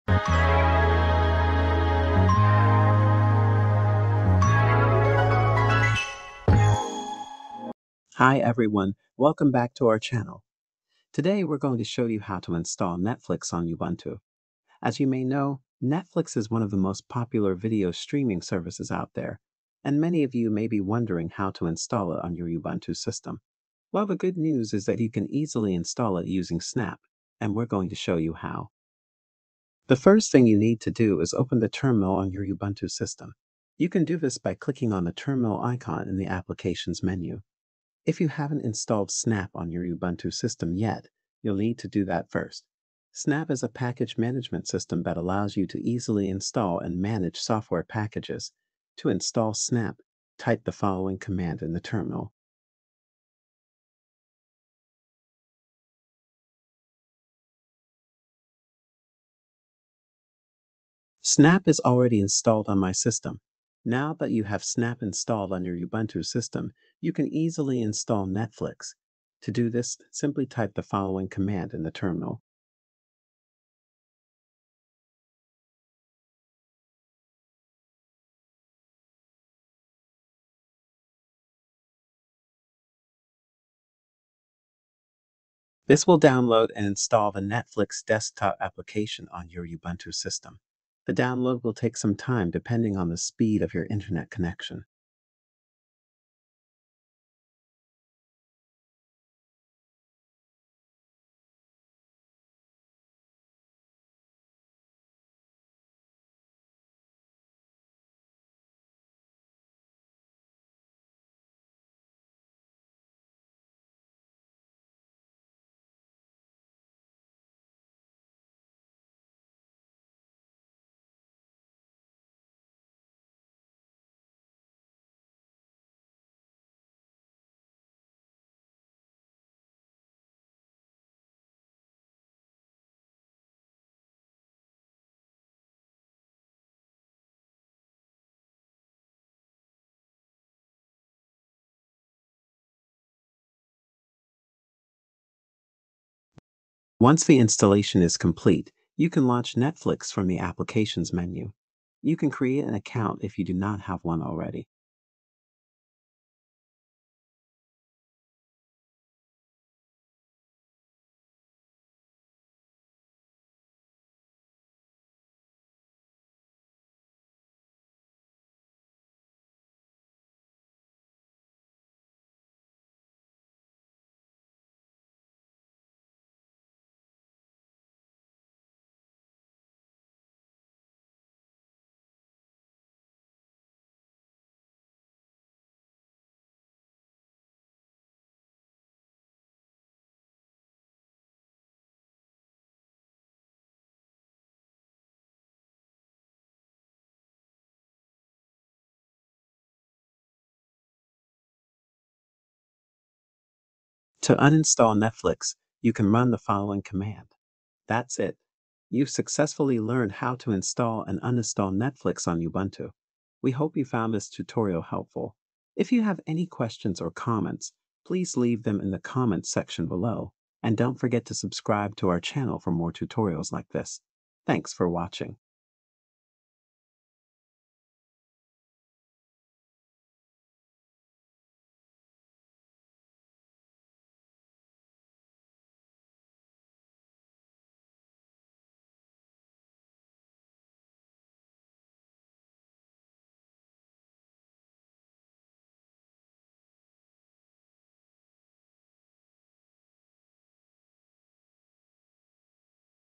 How to Install Netflix on Ubuntu 22.04
In this video i will show you how to install Netflix on Ubuntu. As you may know, Netflix is one of the most popular video streaming services out there, and many of you may be wondering how to install it on your Ubuntu system. Well, the good news is that you can easily install it, and we're going to show you how.